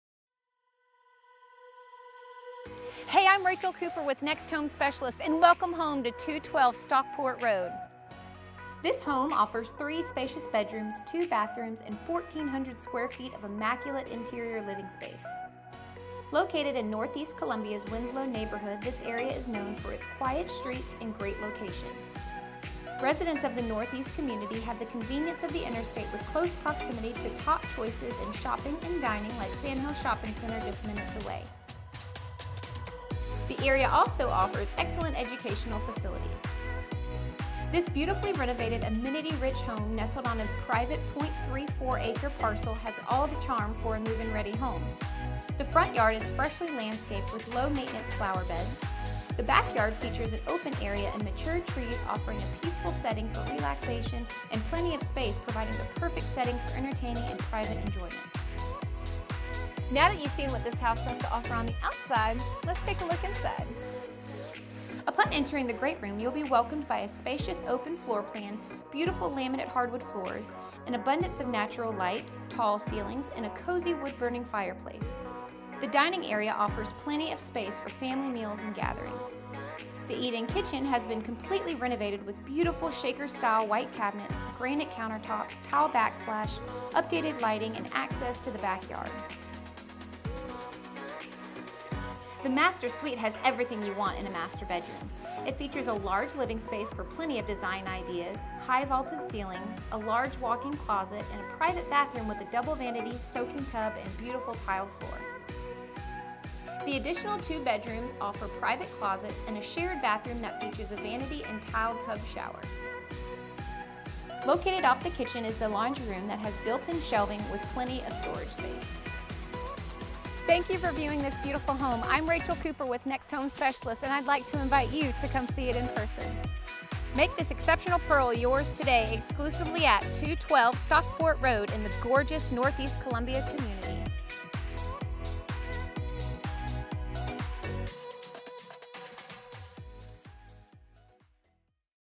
212 Stockport Road - Virtual Tour
This beautifully renovated 1400 square foot, 3 bedroom 2 bath home in the sought after Northeast Columbia community offers all the charm for a move in ready home! ** Offered at just $155,000 **
👈👈 Follow the link for more photos/info

✔ Completely Renovated Kitchen
✔ Tile Backsplash & Granite Countertops
✔ Stainless Steel Appliances
✔ Beautiful Laminate Flooring